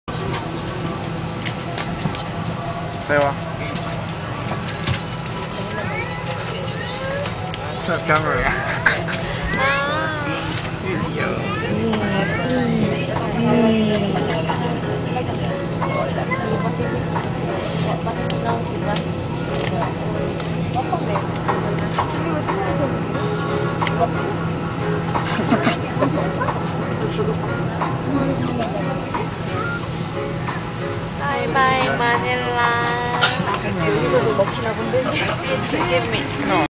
Departing NAIA
Heading back to Cebu after a refreshing trip to Makati